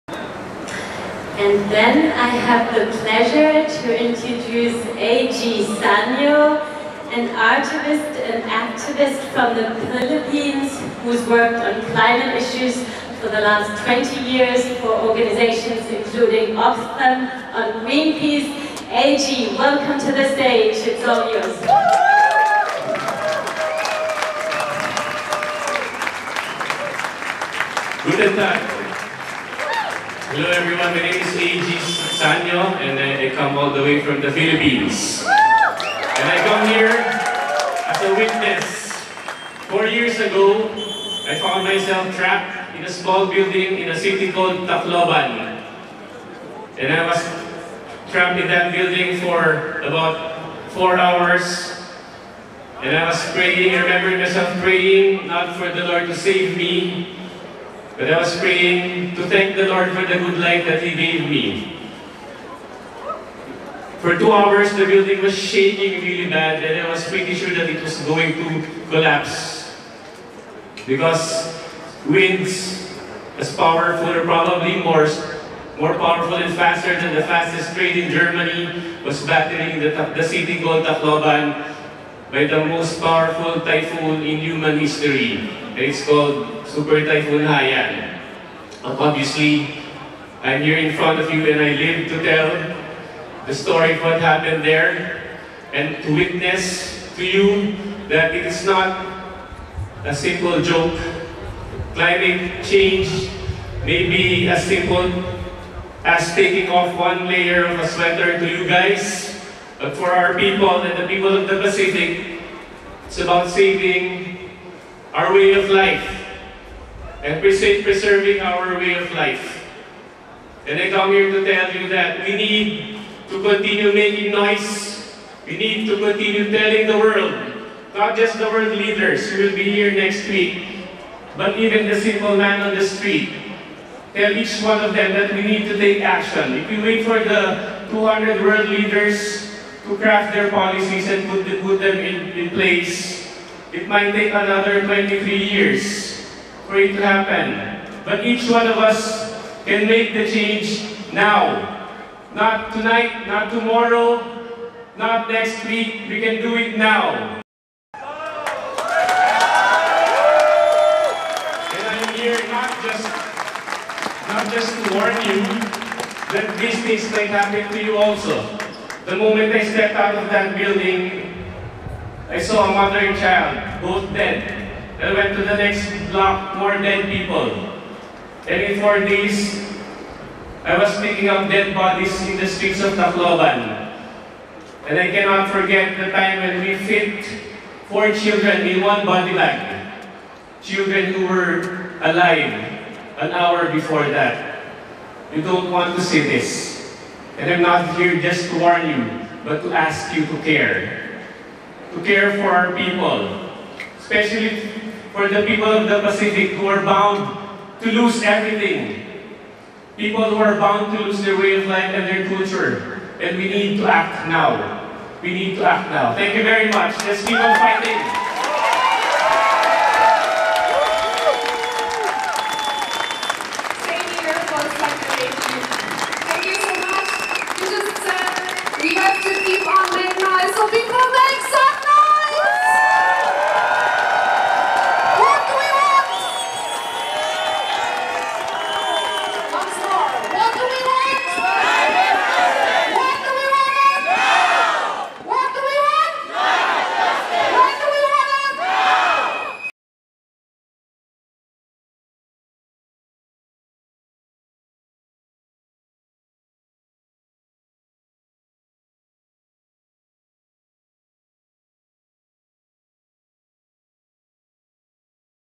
Eachone of us can make the chage now! (A.G. Saño, Philippines)
Bonn, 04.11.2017. Final rally for the demo of the World Climate Conference COP23: Protecting the climate - stopping coal! Red line against coal!